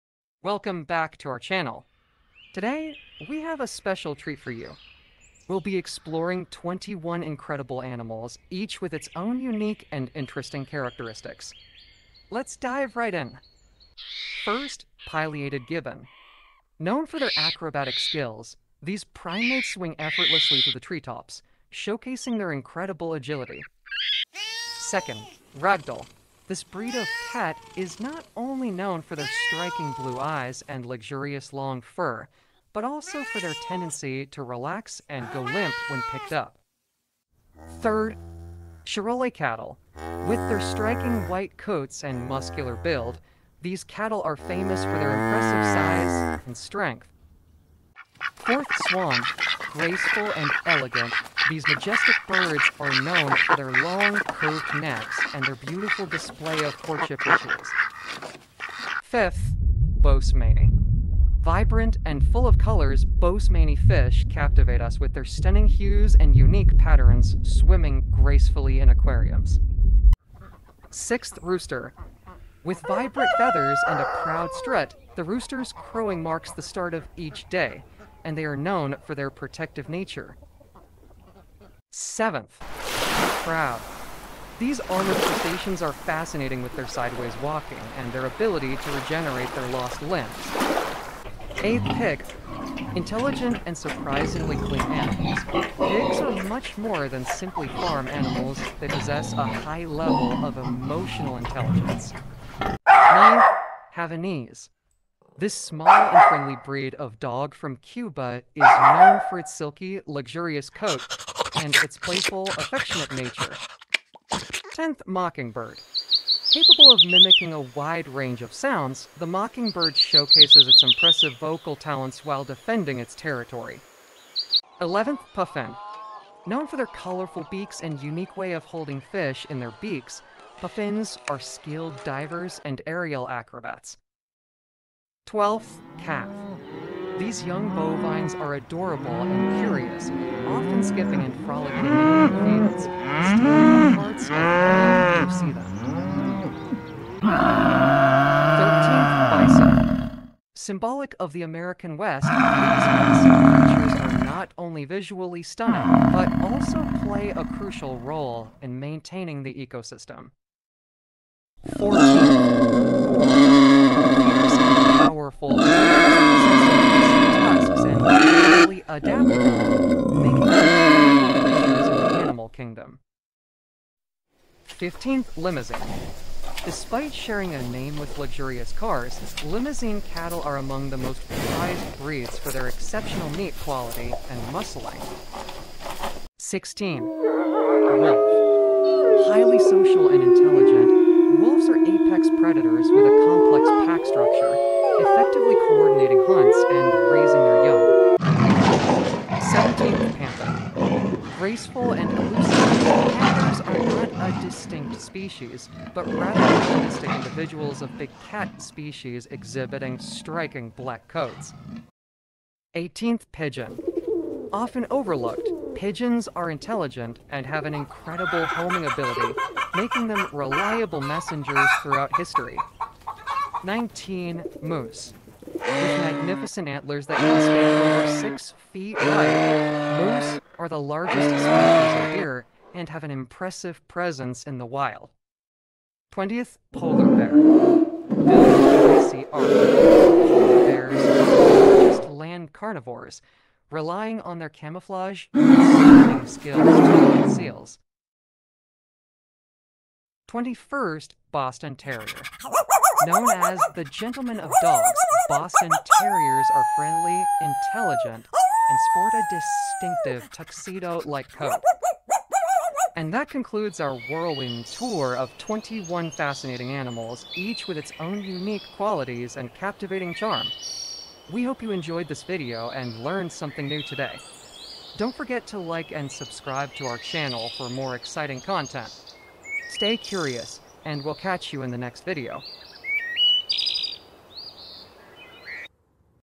Fascinating Facts about Unique Animals: Cattle, Dog, Pig, Bird, Wolf, Rooster
Fascinating Facts about Unique Animals: Cattle, Dog, Pig, Bird, Wolf, Rooster - Animal Sound

In this animal video, let's learn about animals & animal sounds around us, like Cattle, Dog, Pig, Bird, Wolf, Rooster, crab, Mockingbird, puffin, and any more animal  videos.

Hopefully, through this video, you can learn about animal sounds, animal videos, animal videos, and animal sound.

We hope you enjoyed this video and learned something new today. Stay curious, and we'll catch you in the next video.